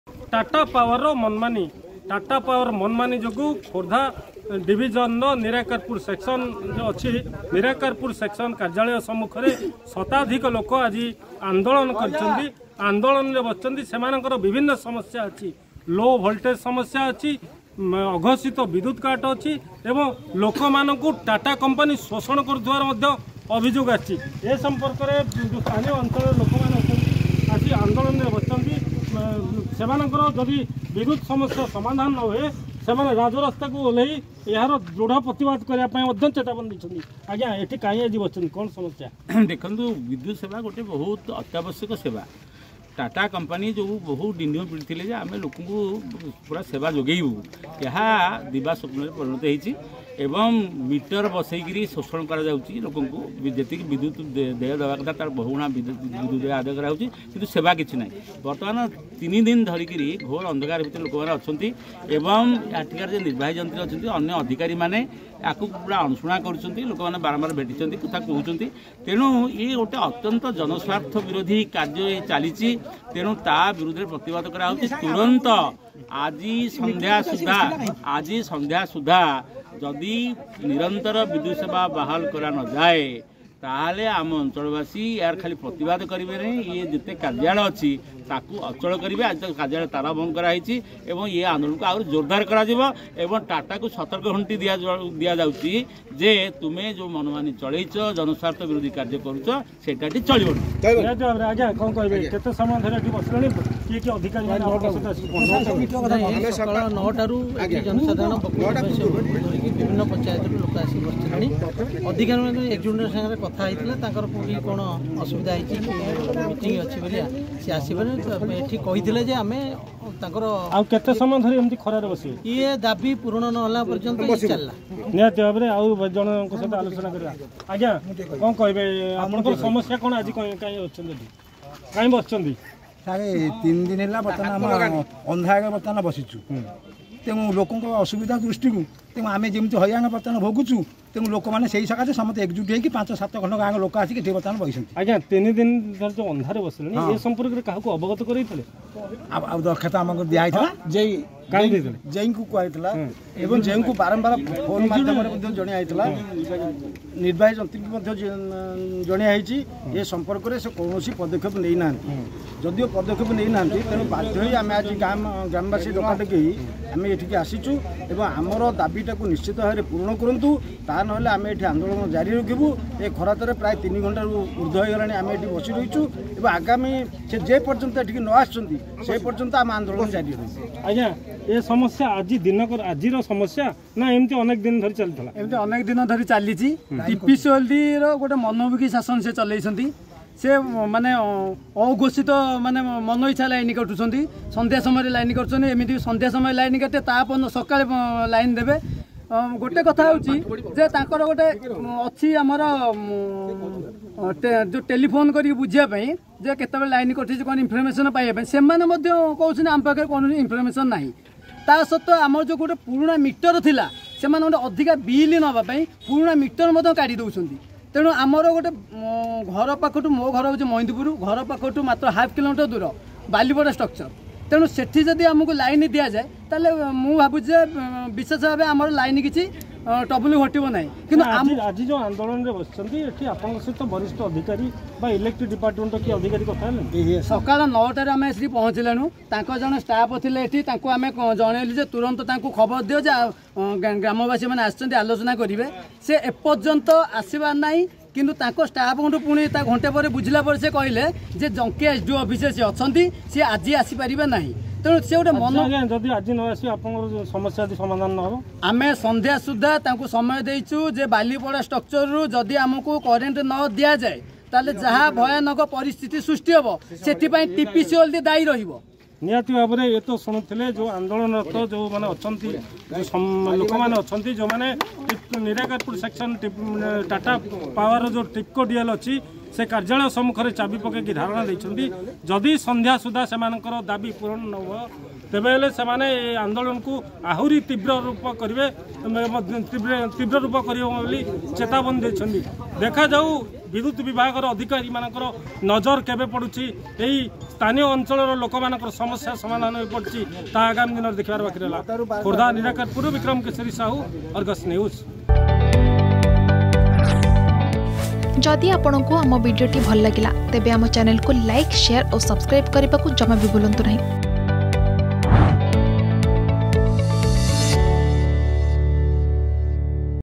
ଟାଟା କମ୍ପାନୀର ମନମାନୀ ନେଇ ଉପଭୋକ୍ତାଙ୍କ ଧାରଣା ଓ ପ୍ରତିବାଦ || TATA Company Issue
ଟାଟା କମ୍ପାନୀର ମନମାନୀ । ଲୋଭୋଲଟେଜ,ଅଘୋଷିତ ବିଦ୍ୟୁତ କାଟ ଓ ବିଦ୍ୟୁତ ବିଲ ଶୋଷଣକୁ ନେଇ ବିଦ୍ୟୁତ ଉପଭୋକ୍ତା ମାନଙ୍କୁ ଶୋଷଣ । ଖୋର୍ଦ୍ଧା ଜିଲ୍ଲା ଜଂକିଆ ବିଦ୍ୟୁତ ଉପଖଣ୍ଡ ନିରାକାରପୁର ବିଦ୍ୟୁତ ସେକସନ କାର୍ଯ୍ୟାଳୟ ମୂଖ୍ୟ ଗେଟରେ ତାଲା ପକାଇ ଗେଟ ସମ୍ମୁଖରେ ବିଦ୍ୟୁତ ଉପଭୋକ୍ତାଙ୍କ ଧାରଣା ଓ ପ୍ରତିବାଦ ।